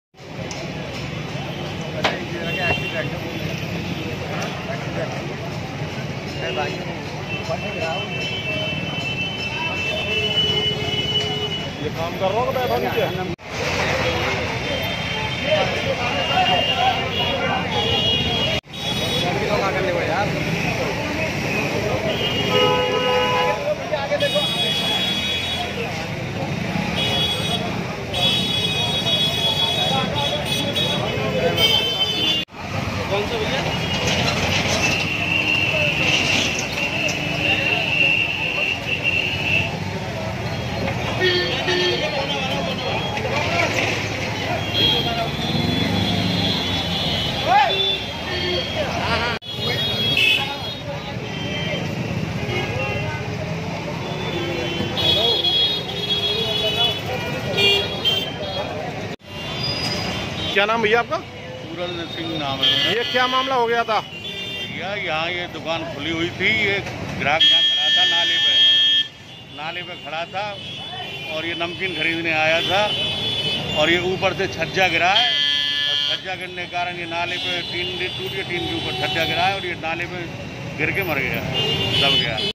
UttarPradesh Mathura news
मथुरा- शहर कोतवाली की कृष्णा नगर पुलिस चौकी क्षेत्र के भूतेश्वर चौराहे पर एक दुकान का छज्जा गिरने से करीब 48 वर्षीय अधेड़ व्यक्ति की दर्दनाक मौत हो गई।